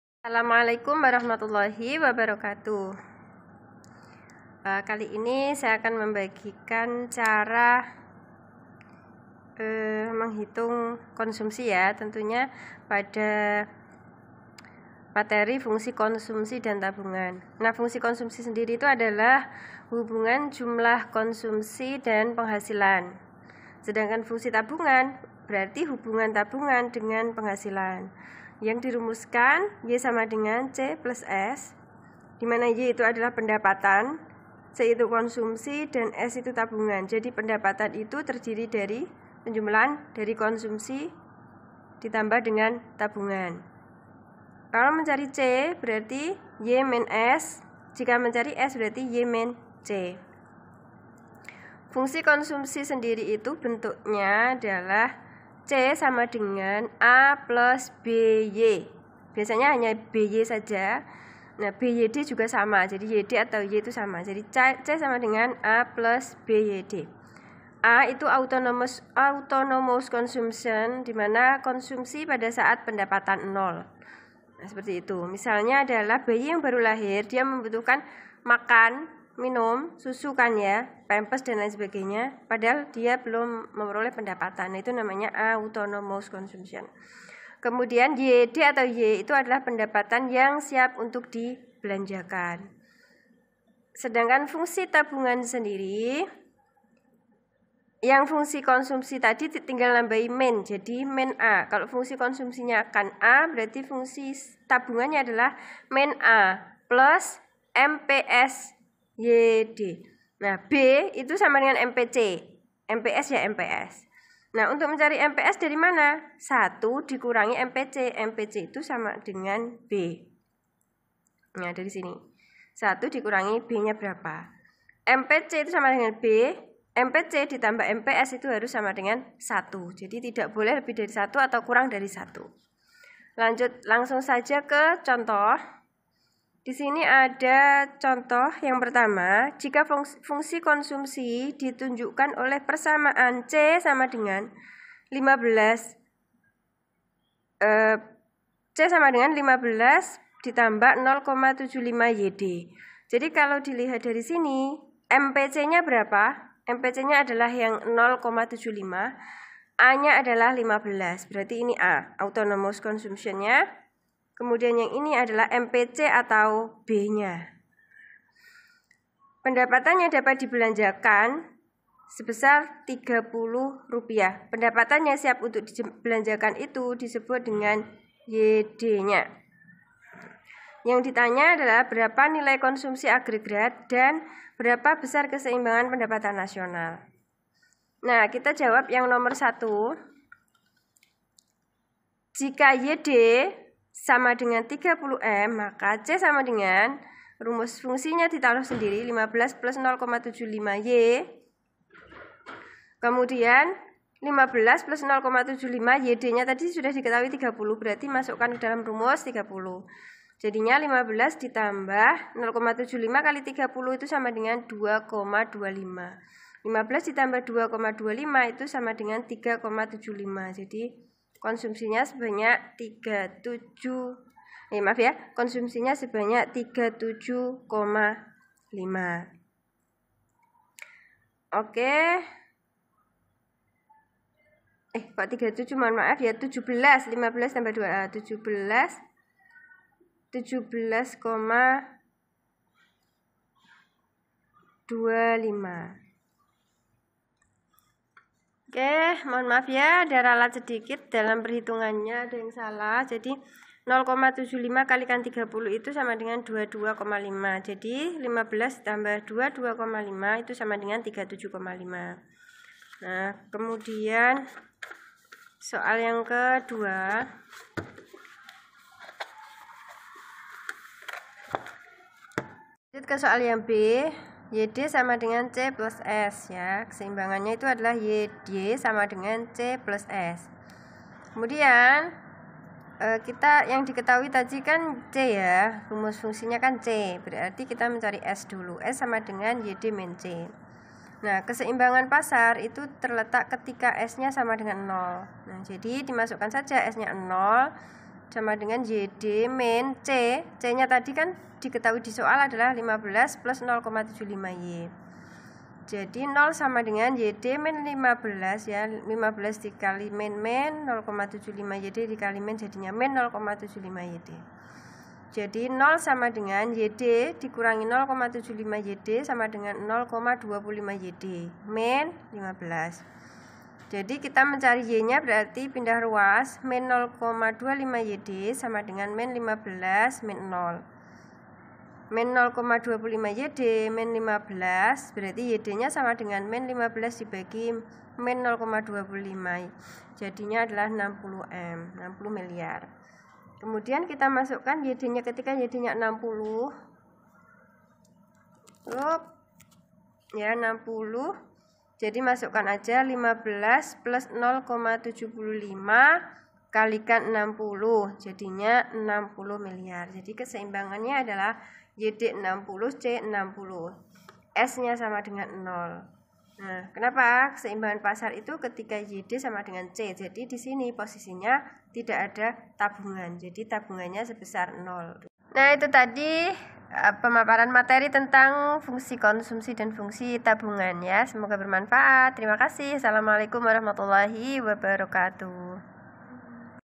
menghitung fungsi konsumsi, fungsi tabungan dan keseimbangan pasar
video ini berisi tentang cara perhitungan fungsi konsumsi, fungsi tabungan, dan keseimbangan pasar